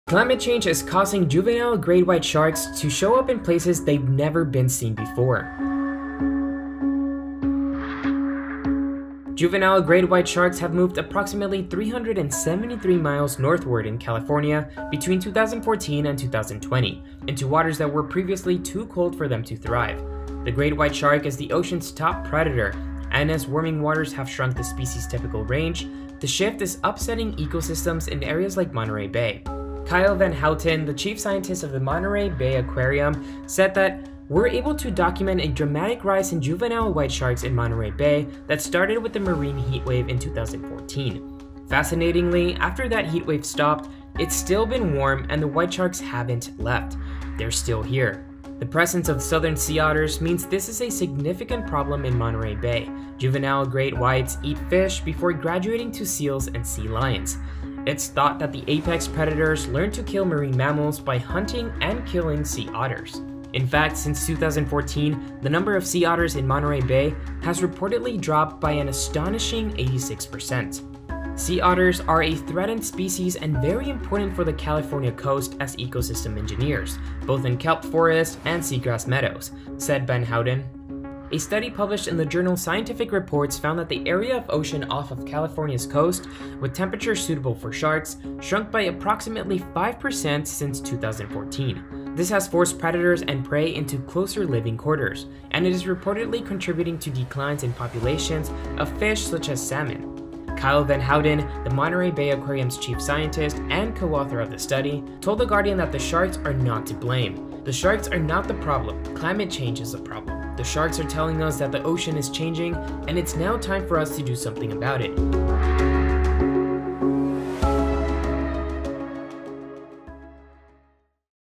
How Climate Change Affects Great White Sharks
Juvenile great white sharks are appearing in places they’ve never been seen before due to climate change — and it's upsetting ecosystems like this one.

Juvenile great white sharks have moved approximately 373 miles north in California between 2014 and 2020 into waters that were previously too cold for them to thrive.

As warming waters shrink the species’ typical range, the shift is upsetting ecosystems in areas like Monterey Bay. It has forced predators and prey into closer living quarters, and it is reportedly contributing to declines in populations of fish such as salmon.